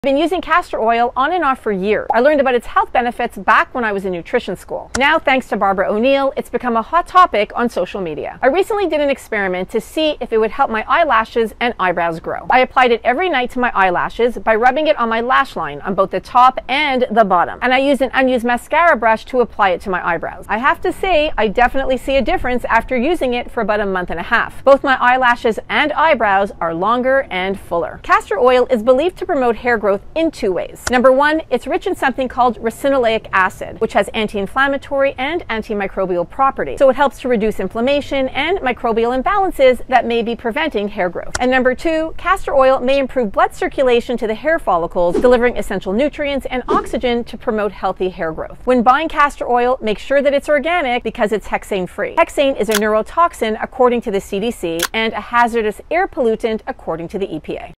Castor Oil: See How It Changed My Eyelashes & Eyebrows!
Have you heard the buzz about castor oil? 🤔 I've been using it for years for liver health, and my recent experiment showed me it works for eyelashes and eyebrows, too.

Castor oil has many health benefits, and it's nice to see how it's become a popular topic on social media.

Have you tried using castor oil? I'd love to know if you found it helpful. Please share. 💙